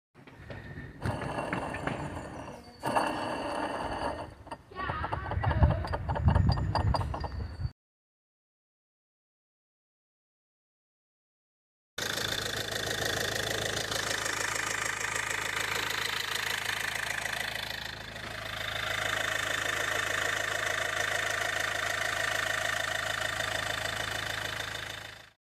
2009 Mk4 2L TDCI Mondeo AC bypass
AC compressor bearings are shot so I decided to bypass it with shorter belt. This was, for me, trial and error as there was no information on routing and most important, belt size. So if anyone else is suffering AC issues on a MK4 mondeo 2L TDCI with PAS pump, here is an, at least, temp solution.